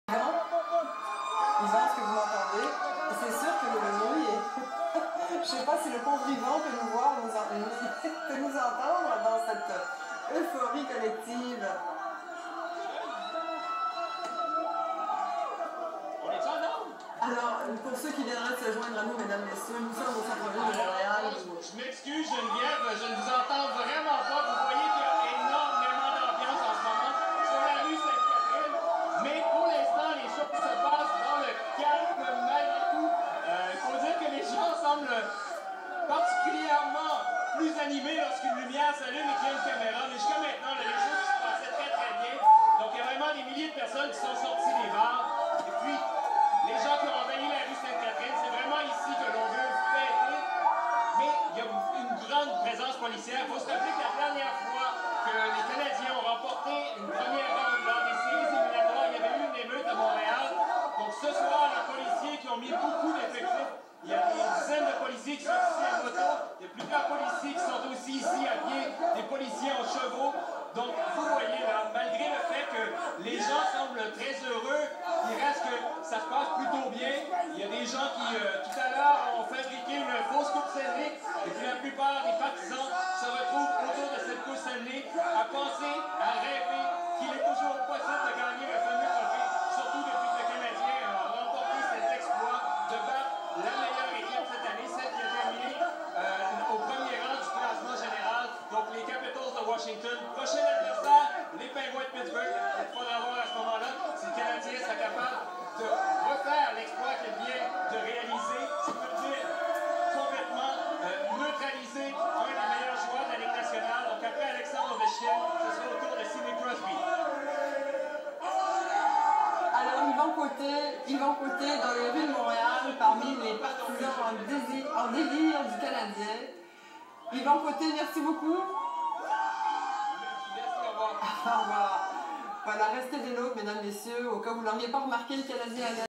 Victoire du Canadiens - RDI en arrache!
Moment live à RDI, après la victoire du Canadiens...